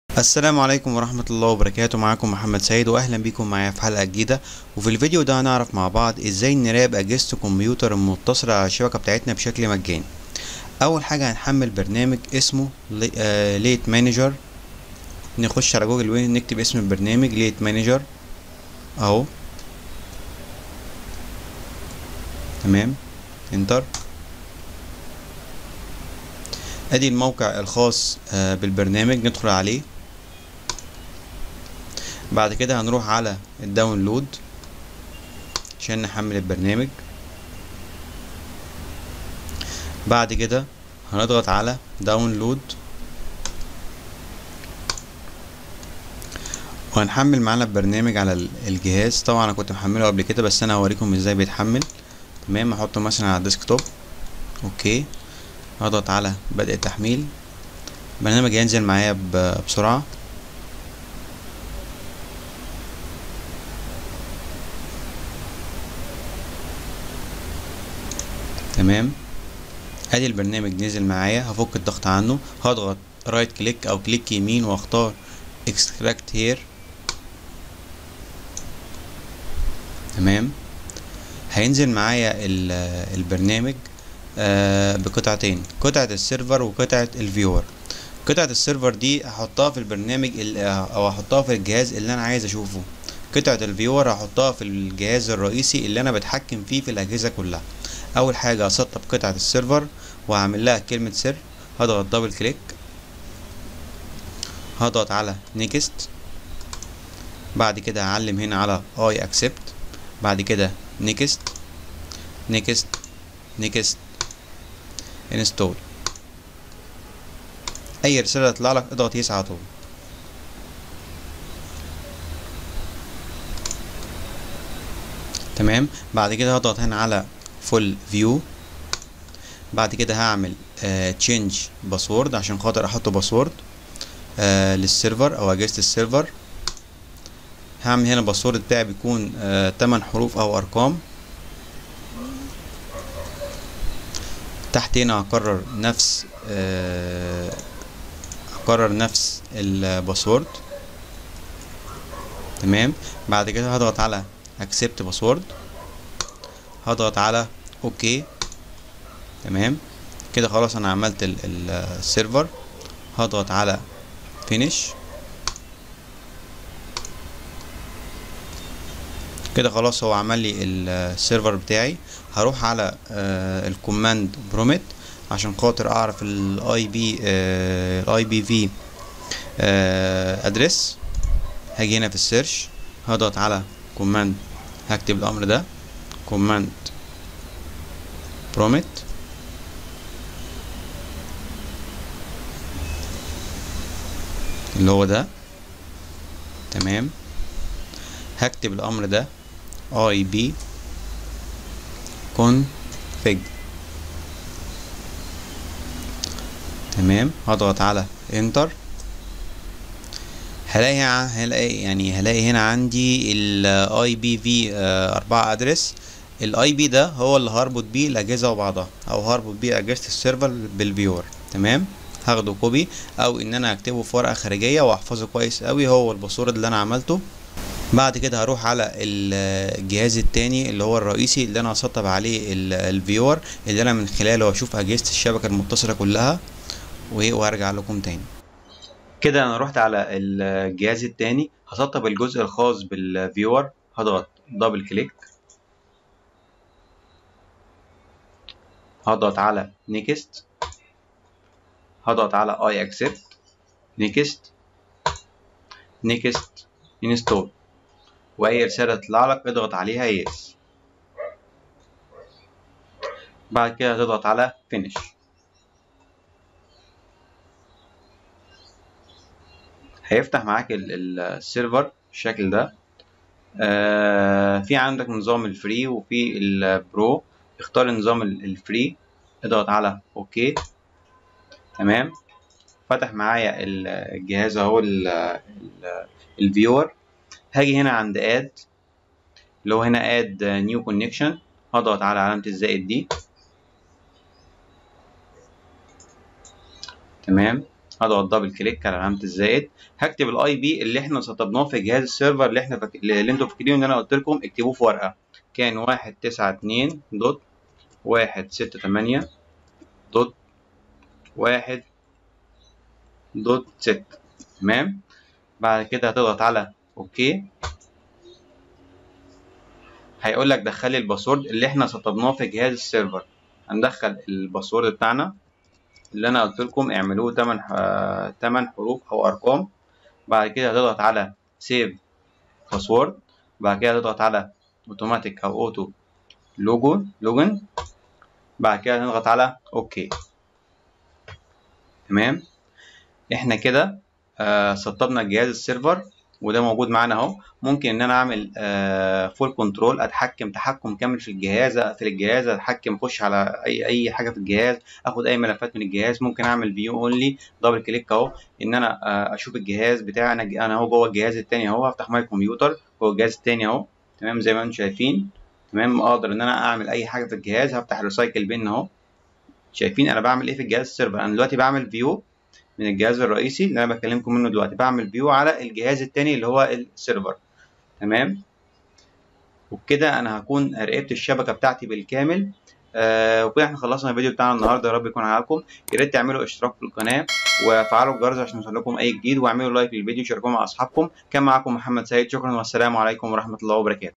تحكم في أجهزة الكمبيوتر داخل الشبكة
تحكم_مجاني_في_أجهزة_الكمبيوتر_في شبكتك_ومراقبة_جميع_الاجهزة
السلام عليكم ورحمة الله وبركاته فى الفيديو ده ياشباب هنعرف مع بعض ازاى نتحكم تحكم كامل وبشكل مجانى فى الاجهزة المتصلة بالشبكة بتاعتنا
ماتنسوش ياشباب تعملوا اشتراك فى القناة ولايك للفيديو.